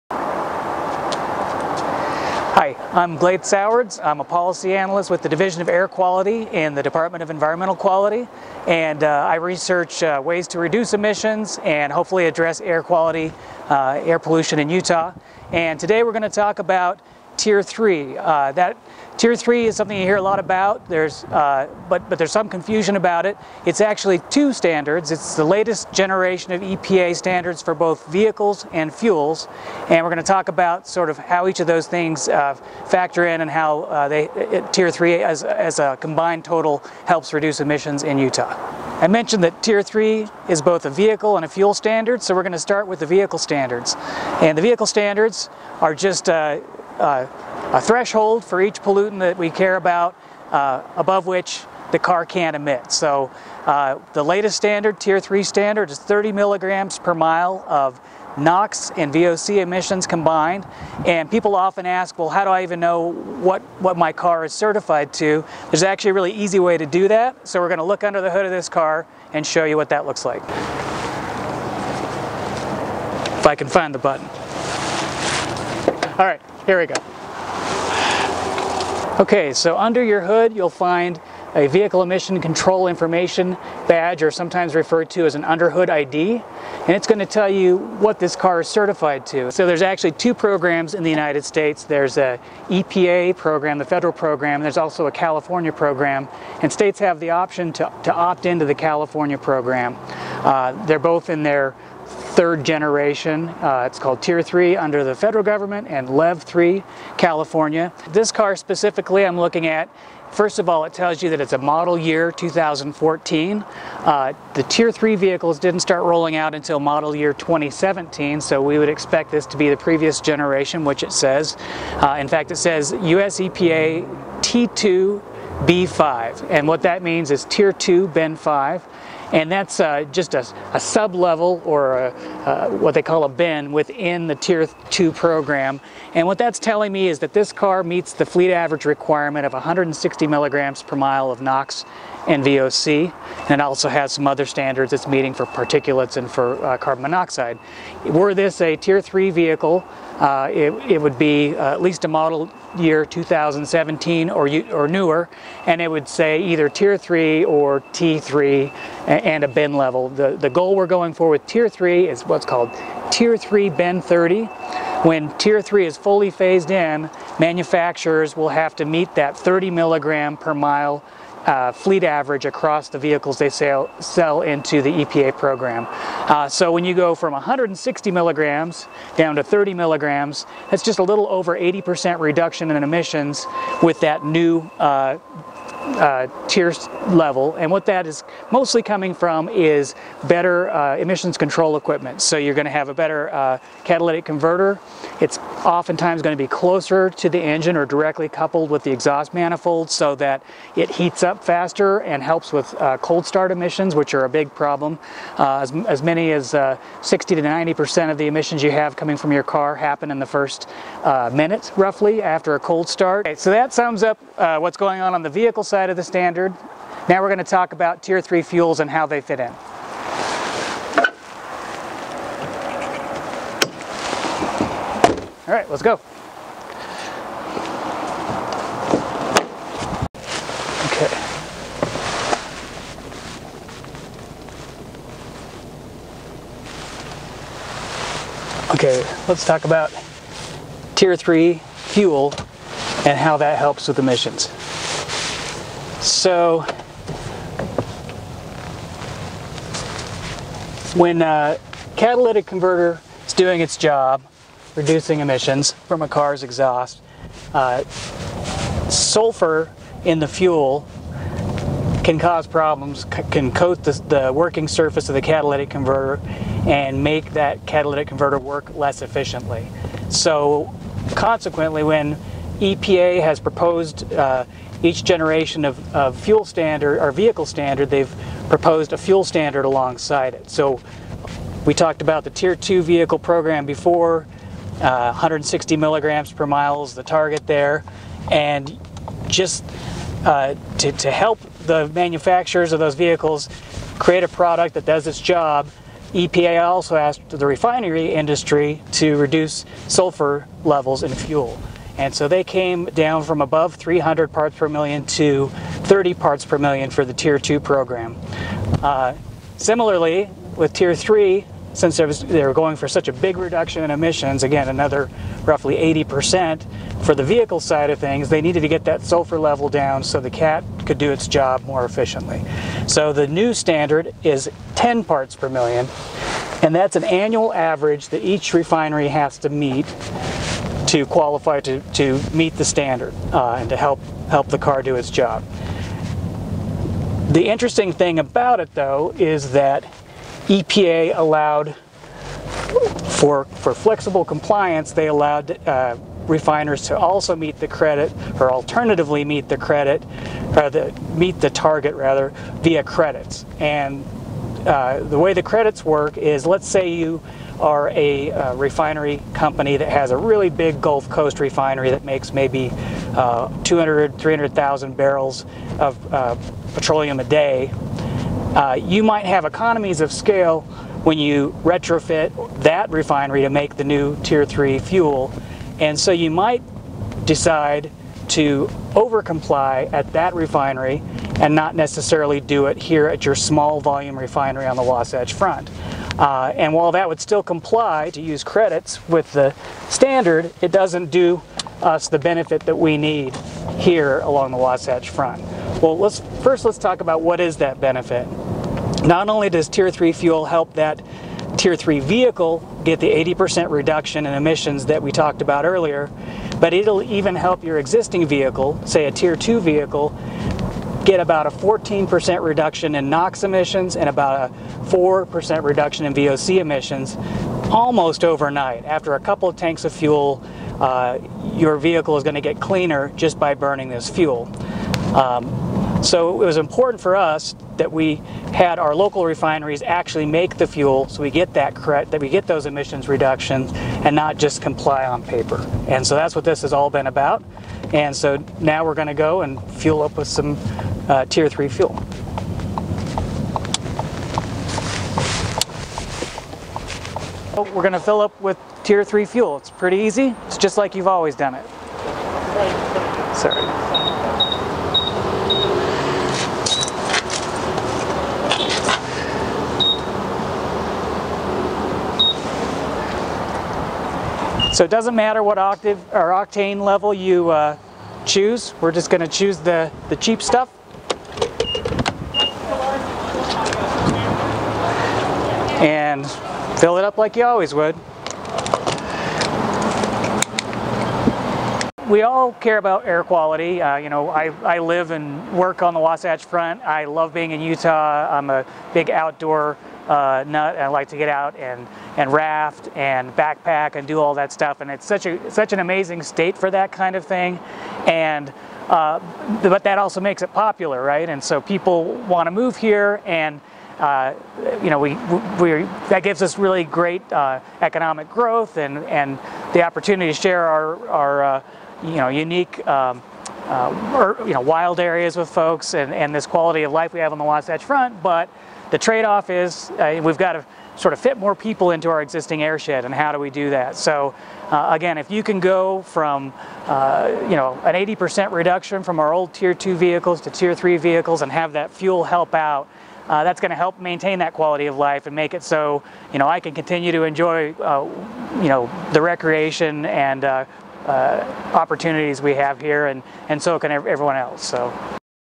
Understanding Tier 3 in Utah
In this week’s installment of IamUtahDEQ video series, we take a ride with Glad Sowards, a policy analyst with Utah’s Division of Air Quality, to get the answers to all your Tier 3 questions. Tier 3 considers the vehicle and its fuel as an integrated system, setting new vehicle emissions standards and a new gasoline standard.